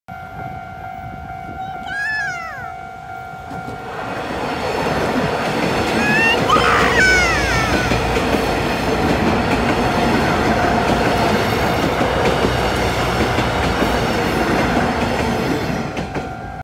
Trainspotting with Hans - Tainan park level crossing 8627
Watch the joy of a 1 year and 11 month old boy watching the train go past.

During their development children go through different phases with very clear interests. Hans has now advanced to the train phase. It is likely that other children will enjoy watching this video.

This video has been recorded as a part of a project studying the development of children created by Ass. Prof. Torbjörn Nordling at the National Cheng Kung University in Tainan, Taiwan.